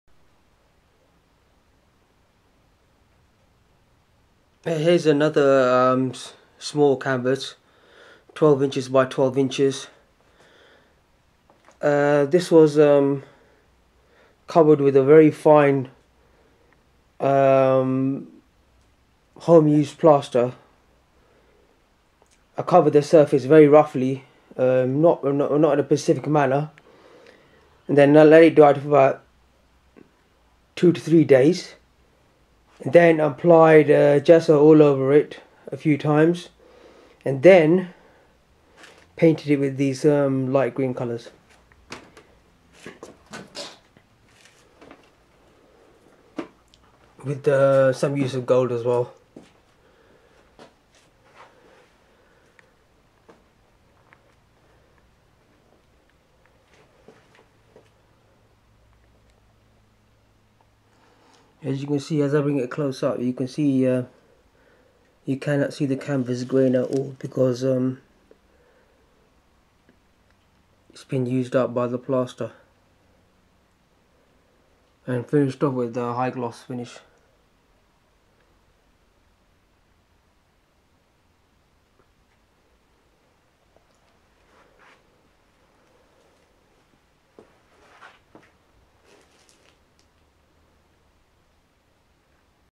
Hand painted small Green abstract canvas in my studio - hardeep s ghatora
Art Studio in west London.
Thanks for viewing my art channel.